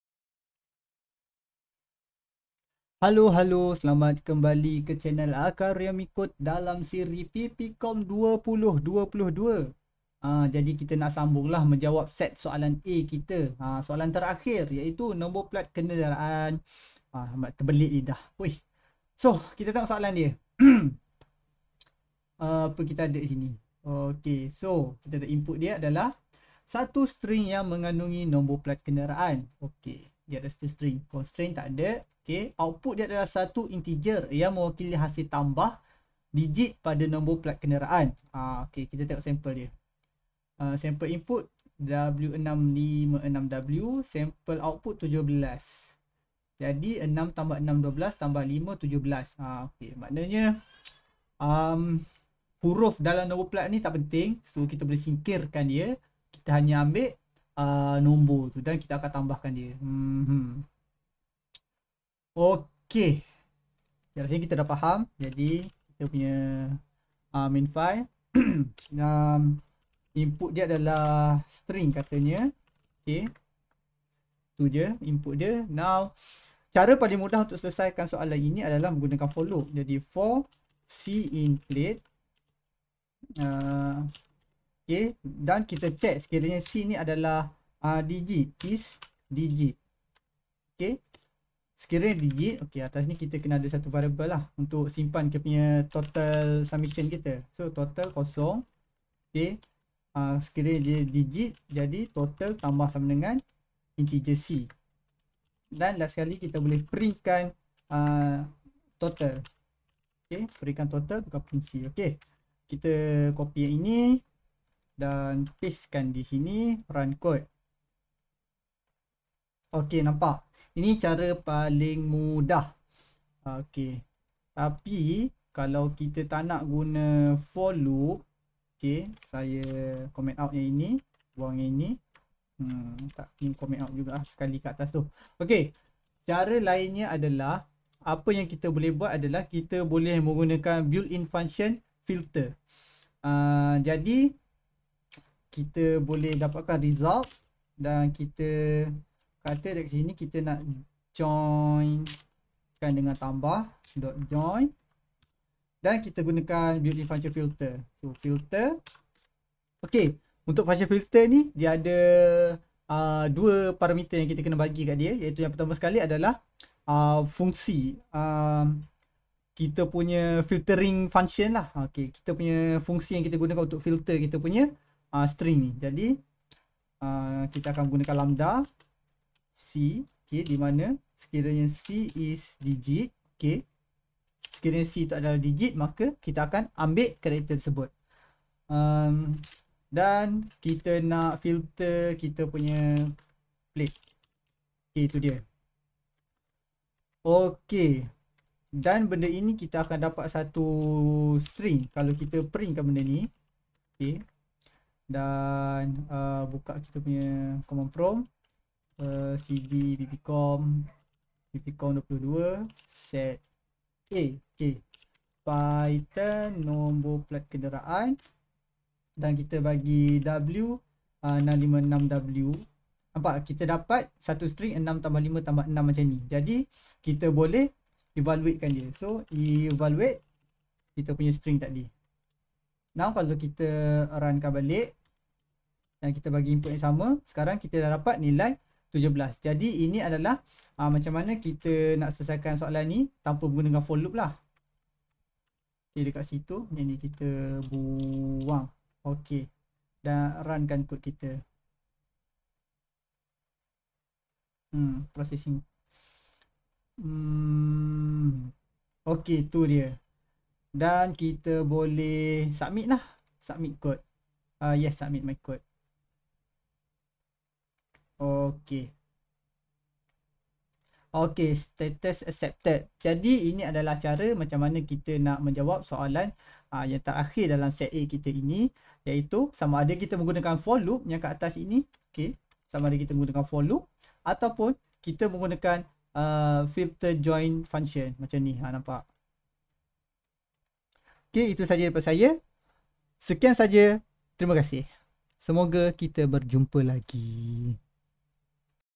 【PPKomp2022】Set A: Soalan 6 - Nombor Plat Kenderaan【Python3】
Hey korang, aku ada join PPKomp2022 and aku nak kongsikan bagaimaa aku mentelesaikan soalan-soalan yang ditaya dalam PPKomp2022.